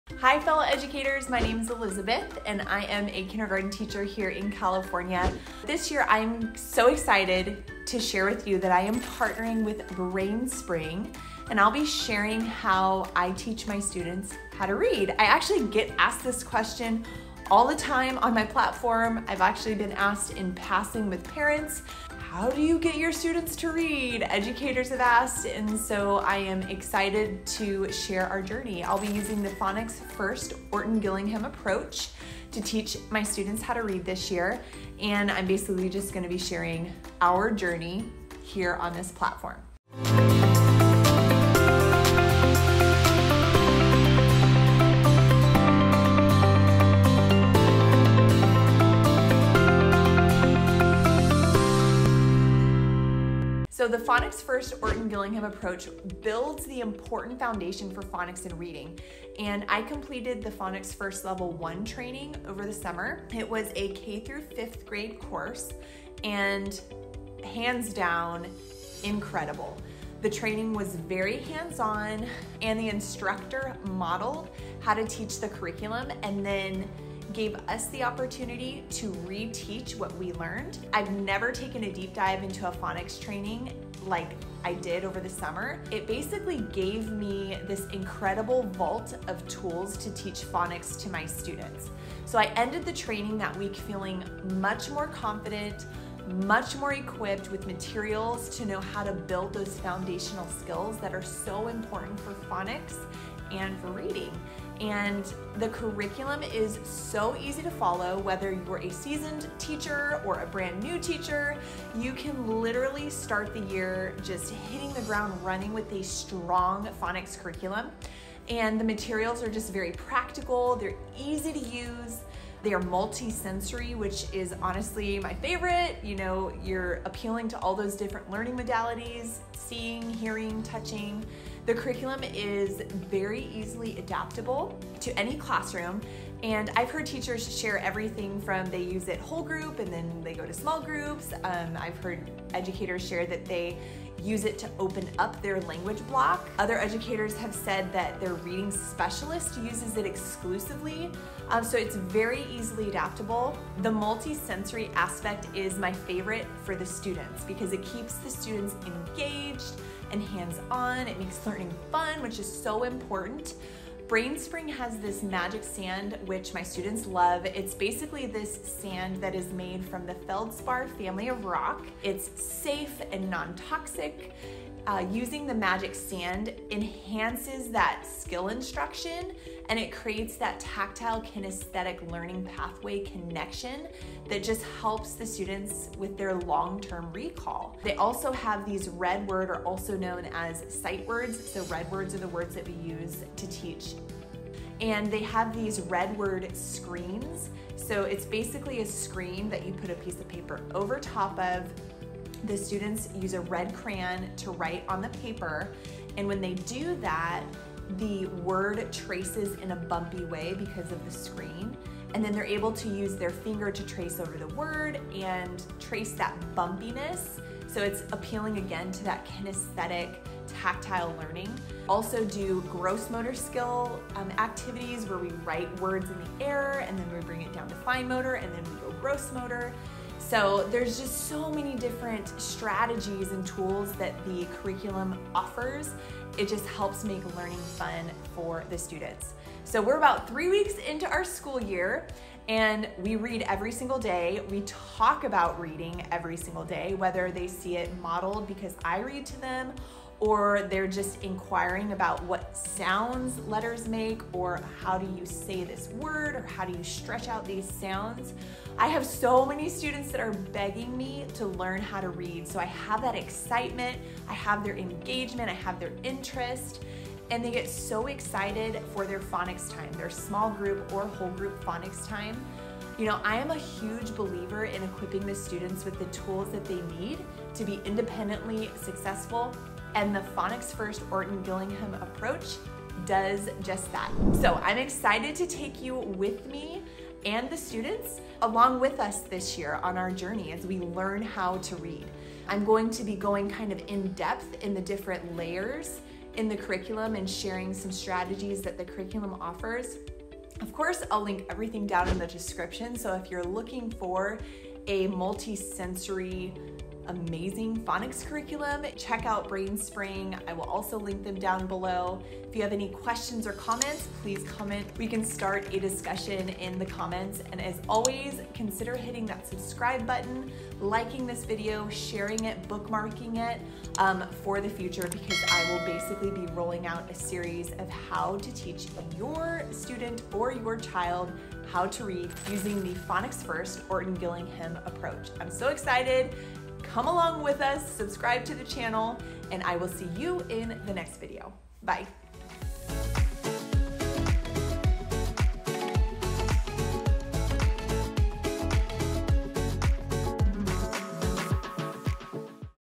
Teach them HOW TO READ using Phonics First with Brainspring | Orton Gillingham Method
Hello educators! In this video, I share the beginning of our journey with Brainspring using the Phonics First Orton Gillingham method to teach my students how to read! We just started the school year and have started whole group and small group phonics instruction. I am partnering with Brainspring for the year and will be sharing our journey for the entire year on how I teach my students how to read!